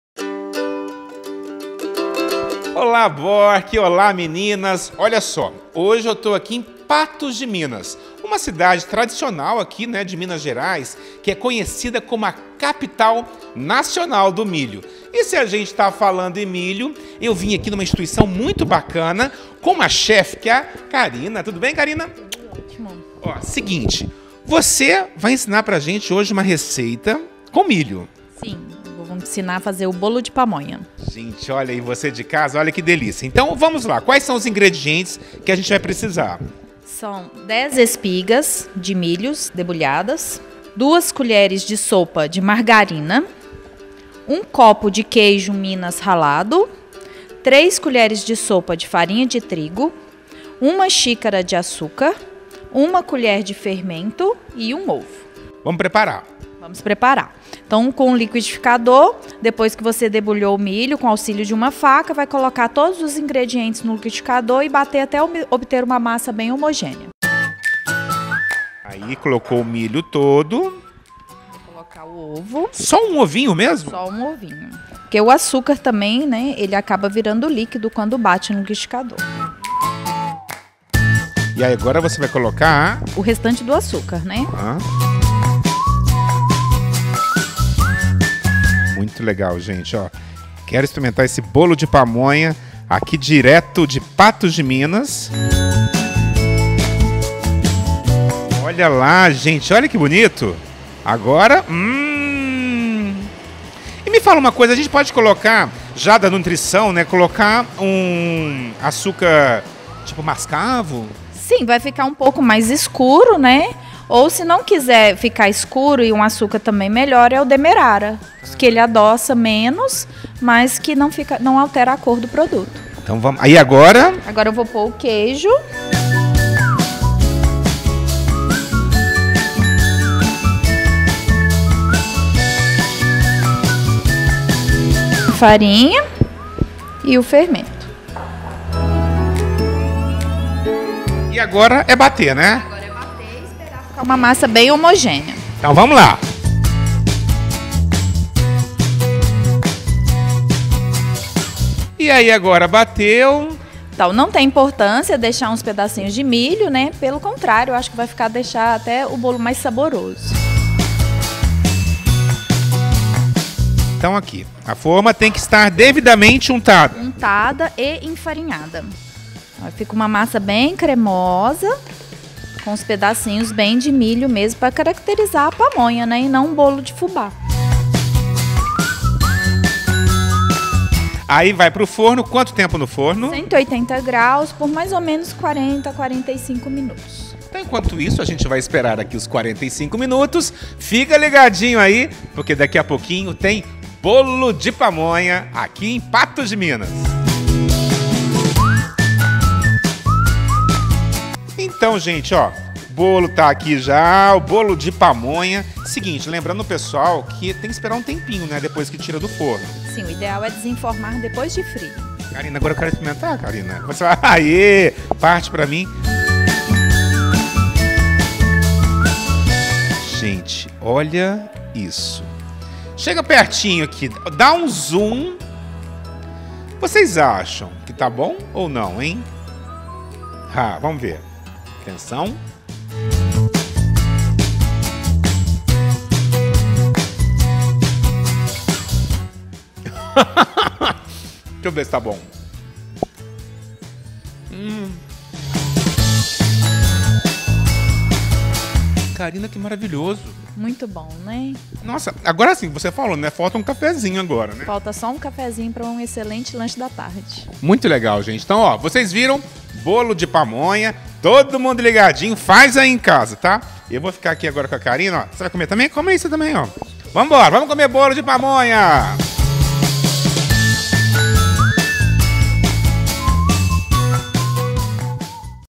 RECEITA BOLO DE PAMONHA
Vamos conferir uma receita super especial, direto de Patos de Minas - MG e especialmente par a Cozinha do Bork. Um delicioso Bolo de pamonha. Quer aprender? Então vem comigo.